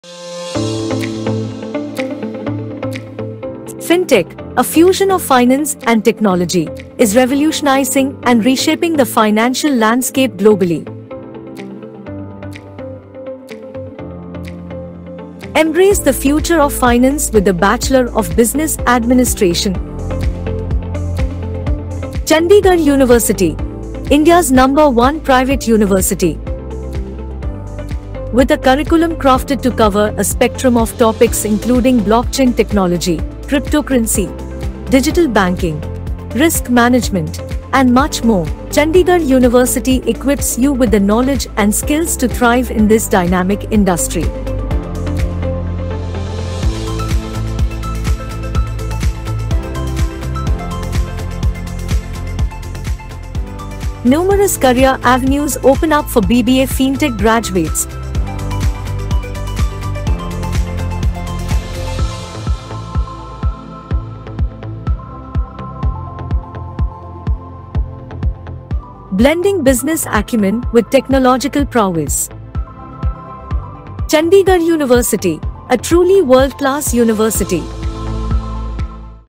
Chandigarh University BBA Fintech Admissions | Scholarships | Placements
Ready to reshape finance through technology's lens?

Combine your passion for finance and tech into a rewarding career in FinancialTechnology with our BBA in Fintech.

BBA Fintech merges financial expertise with technological advancements, creating disruptive and innovative solutions for the finance industry. Dive into thematic investing, digital currency, financial markets, cybersecurity, education lending, and credit scoring with our comprehensive curriculum.
Position yourself as a sought-after asset for leading banks and Big Tech companies. Seize the opportunity to join the BBA in Fintech Programme at NAAC A+ Chandigarh University, India's No. 1 QS World Ranked Private University.

Apply now and unlock the gateway to a promising future in the realm of Financial Technology.

Register via CUCET to avail of up to 100% scholarship worth Rs 170 Crores